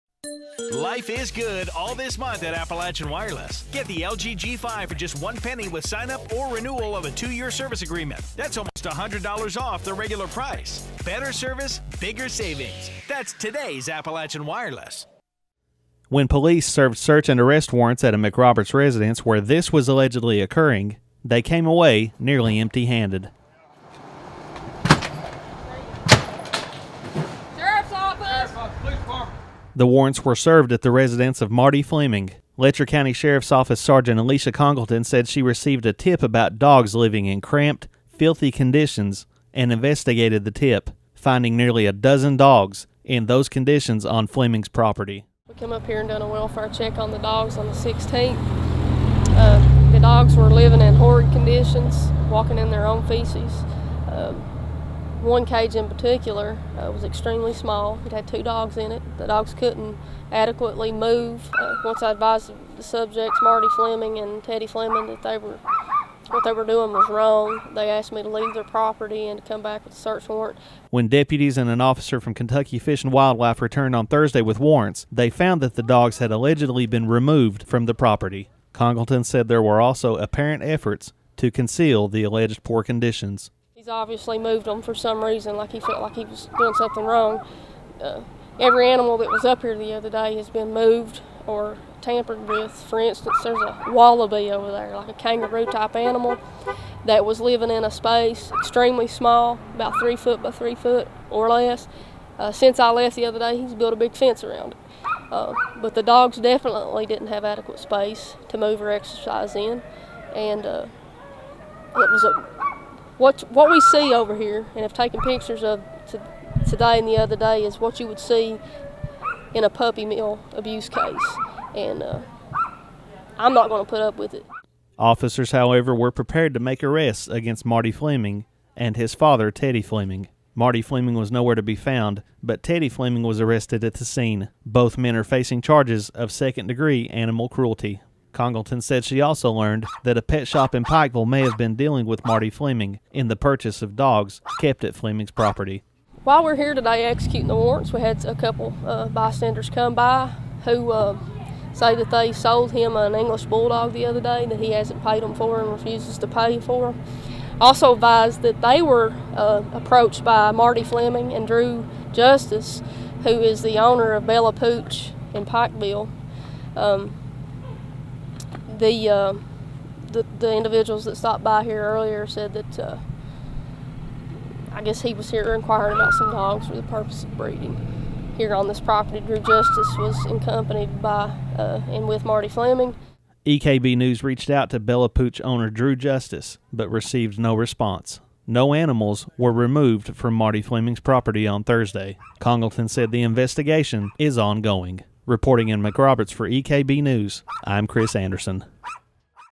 Alleged puppy mill investigation underway in Letcher County
Police in Letcher County investigating an alleged puppy mill came up short Thursday afternoon when they served search and arrest warrants, but that didn’t stop one sheriff’s deputy from describing what she allegedly saw just days before. EKB News Reporter Chris Anderson was at the scene when the search warrant was served. He has this report.